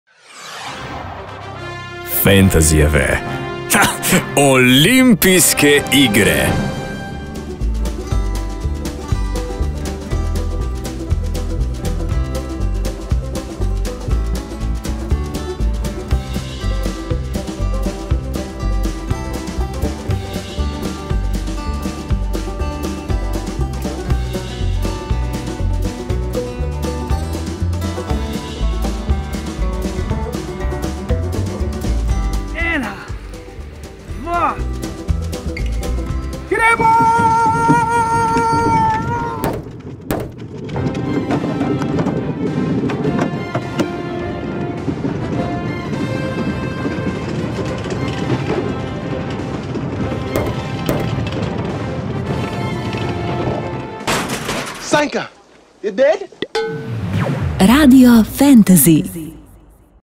Bob ekipa, da te skipa
Fantasyjeva reprezentanca v bobu je danes imela precej pester dan. Janko in Mato sta kljub težkim vremenskim pogojem uspela navdušiti občinstvo v Fantasyjevi deželi.